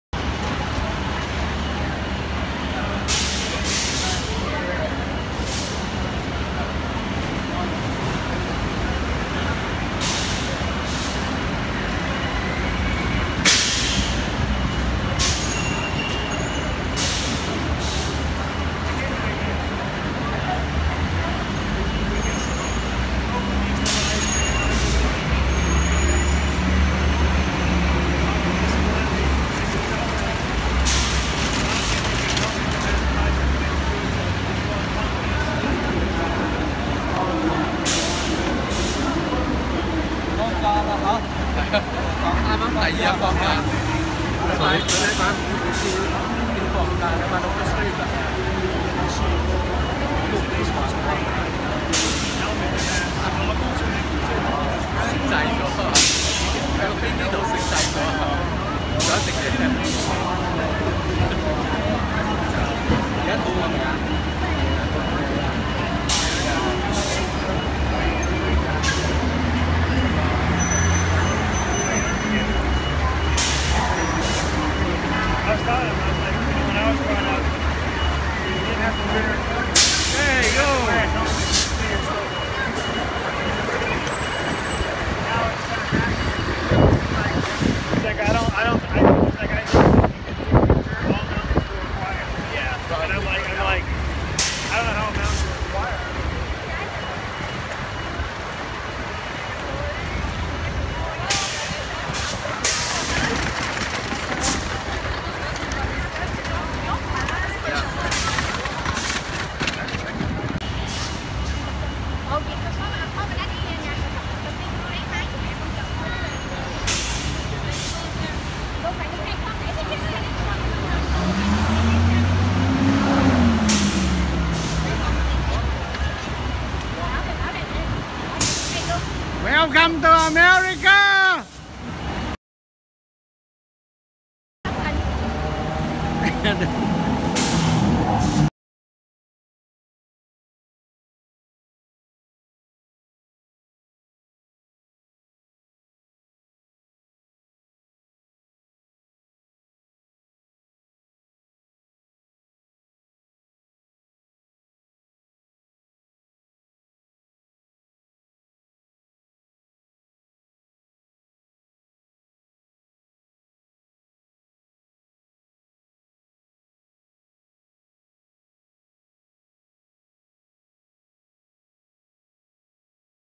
2018 cô rùa lật ngữa đã bò tới Mỹ bình an Los Angeles Tom Brady International Airport
Day 1 Welcome Auntie Trịnh Thị Hảo to California USA cô rùa lật ngữa đã bò tới Mỹ bình an LAX